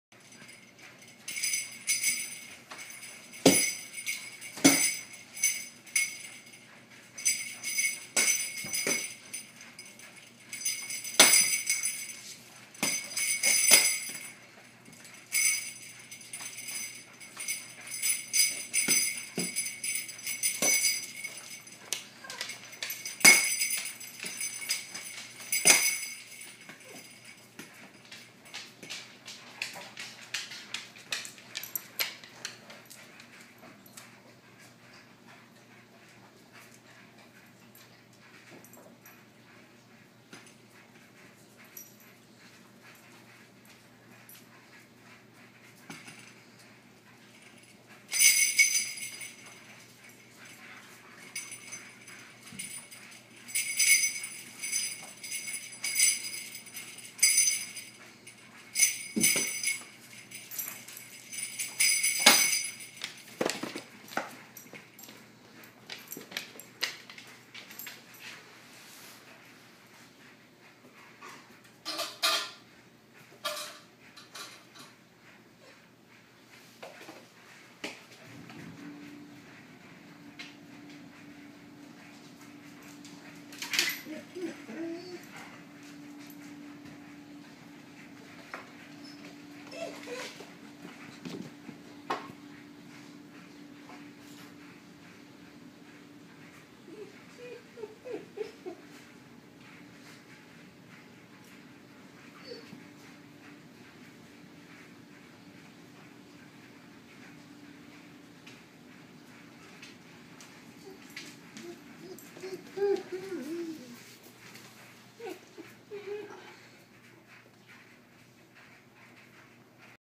Zoe learns to ring the bell
Shiloh Shepherd pup telling us she needs to go outside.